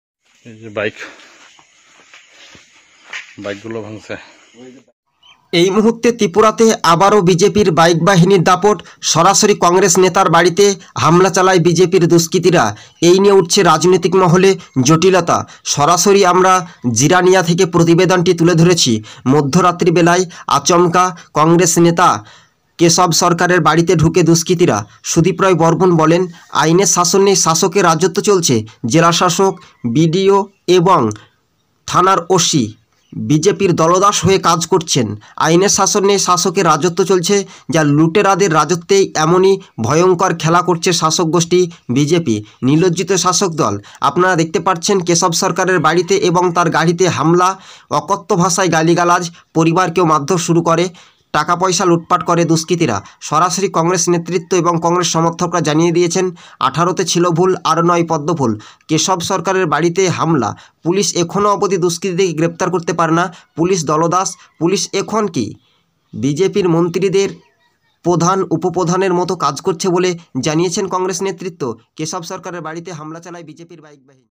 সুদীপ রায় বর্মন অনুগামীর বাড়িতে হামলা মধ্যরাতে টাকাপয়সা লুট করে বাইক বাহিনী বিজেপি!
কেশব সরকারের বাড়িতে তান্ডব দুষ্কৃতীদের, কংগ্রেস করার অপরাধে মধ্যরাতে হামলা! প্রতিবেদনটি জিরানিয়া থেকে?